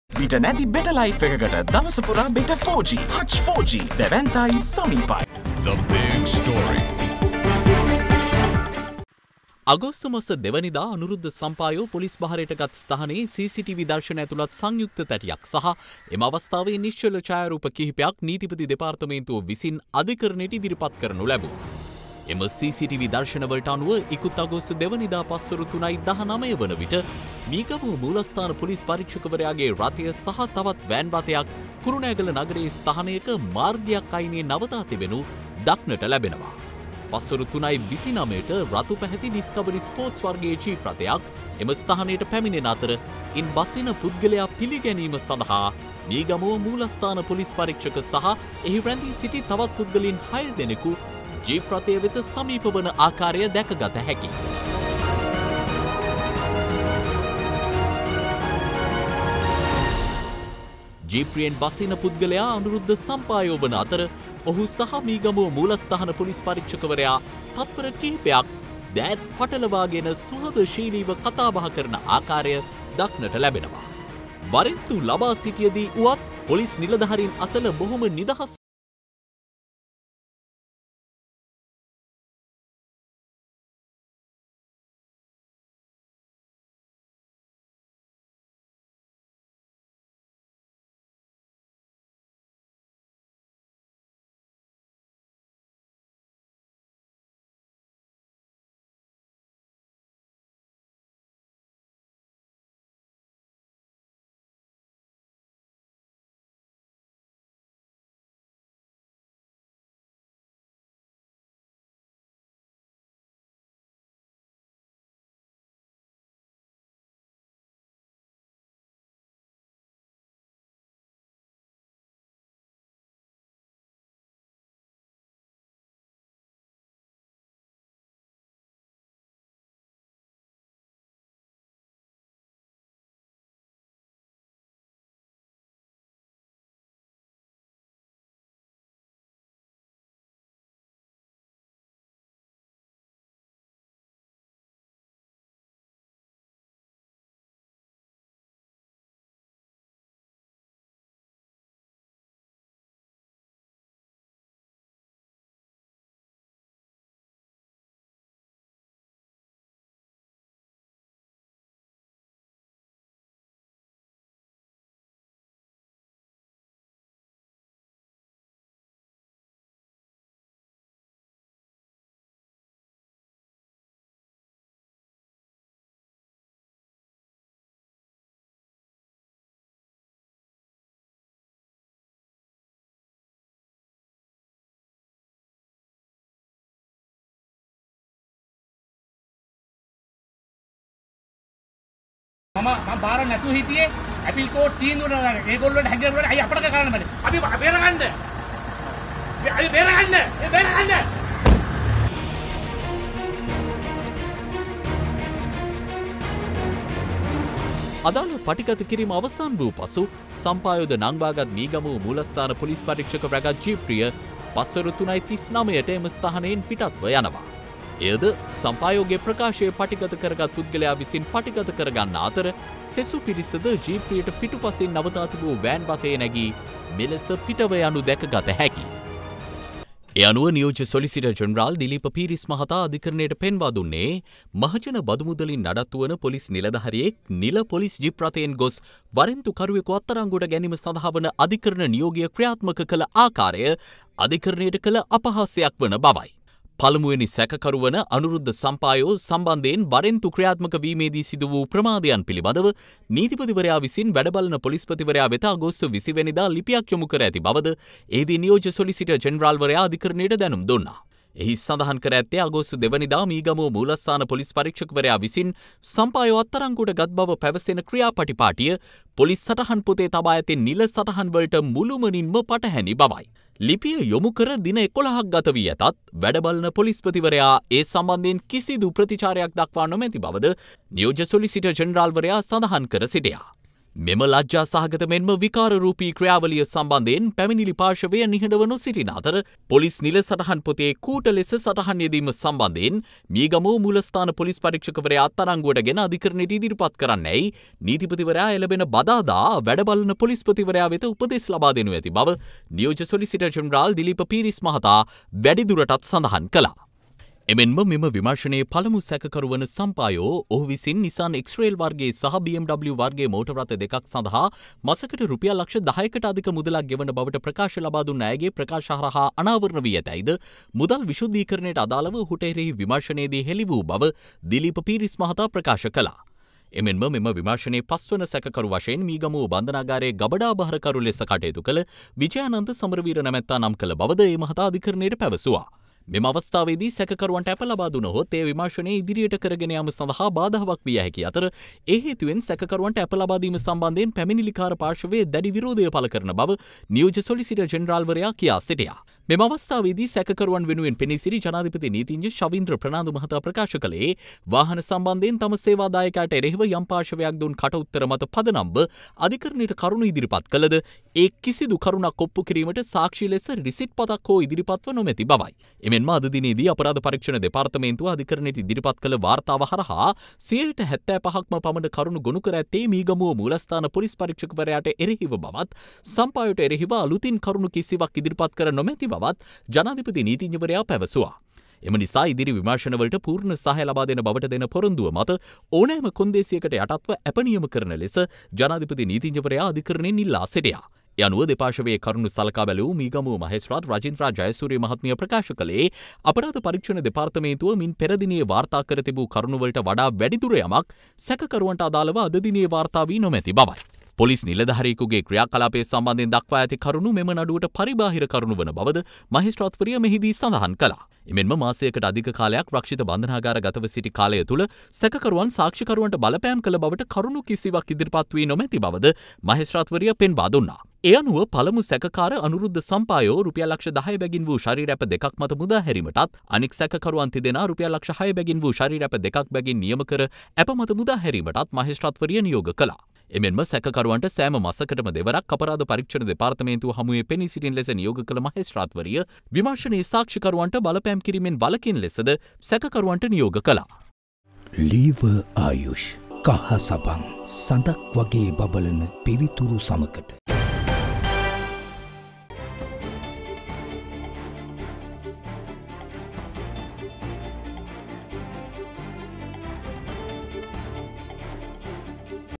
සම්පායෝ සැබෑවටම අත්අඩංගුවට ගත් හැටි වීඩියෝ සාක්ෂියකින් එළියට
CCTV footage of Sampayo's arrest released